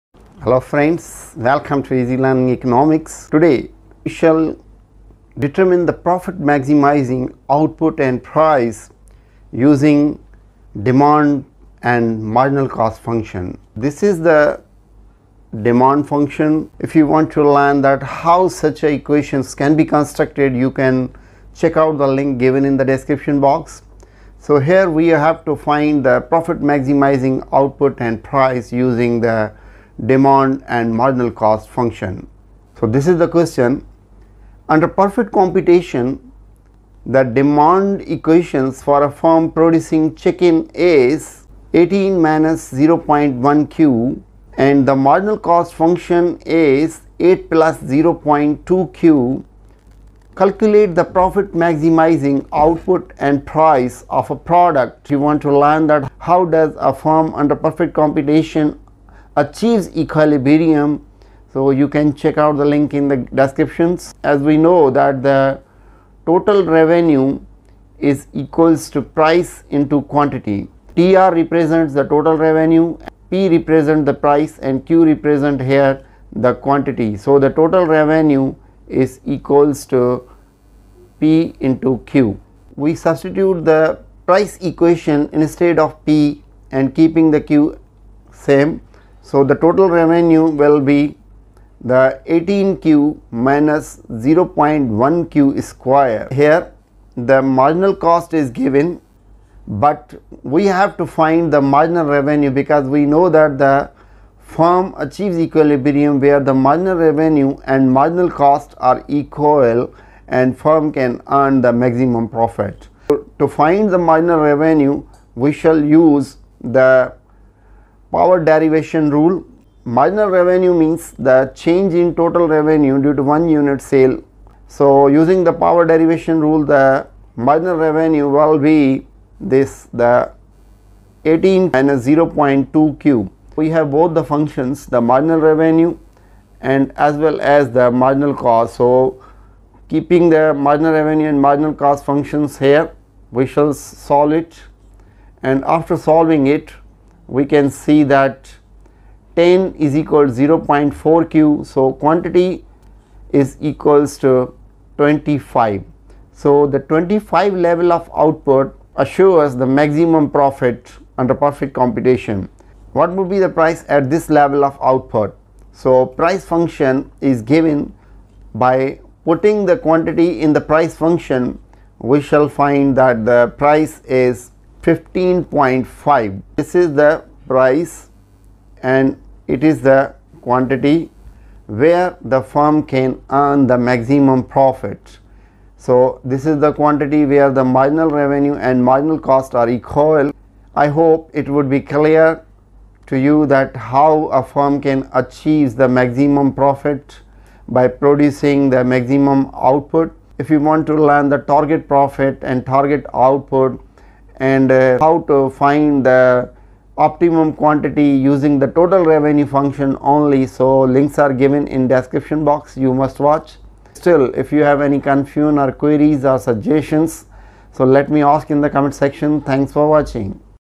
136- Using Demand Equation and Marginal cost Function calculate Profit maximizing Output
Using Demand Equation and Marginal cost Function calculate Profit maximizing Output

Cost Analysis with Calculation of cost, their relationship in the short run Period. Video no: 44